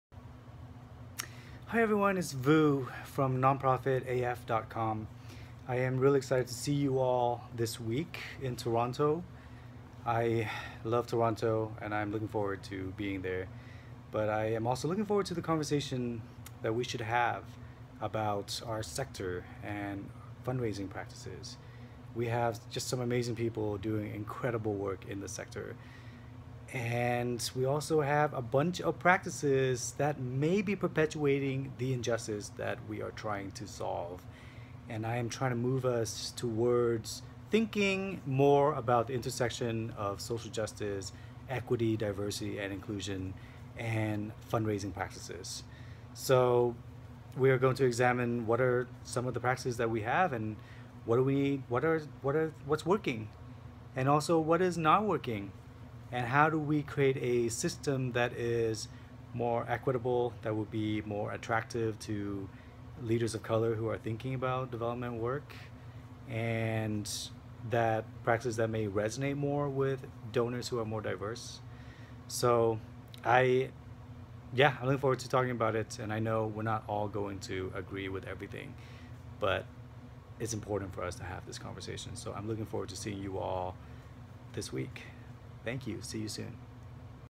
AFP Leadership Academy Message from Open Plenary Speaker Vu Le
Vu Le prepares us for his Opening Plenary talk on Friday with some questions about our practice and how we can better approach our missions and goals to effectuate the positive change we want.